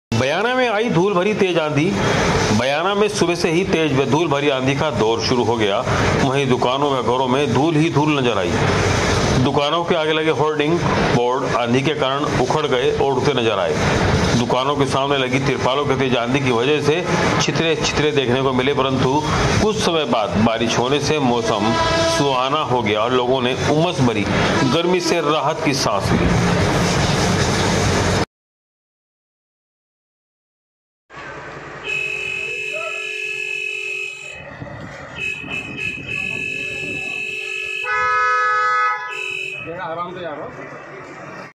में सुबह से ही तेज एवं धूलभरी आंधी# का दौर#gnewsindia#rajsthan#
बयाना ।

बयाना में आई धूलभरी तेज आंधी ।

बयाना बयाना में सुबह से ही तेज एवं धूलभरी आंधी# का दौर शुरू हो गया । वहीं दुकानों एवं घरों में धूल ही धूल नजर आई । वहीं दुकानों के आगे लगें होडिंग वोंड आंधी के कारण उखड़ गए एवं उड़ते नजर आए । दुकानों के सामने लगीं तिरपालों के तेज आंधी की वजह से छितरे छितरे देखने को मिलें। परन्तु कुछ समय बाद बारिश होने से मौसम सुहावना हो गया । और लोगों ने उमंस भरी गर्मी से राहत की सांस ली ।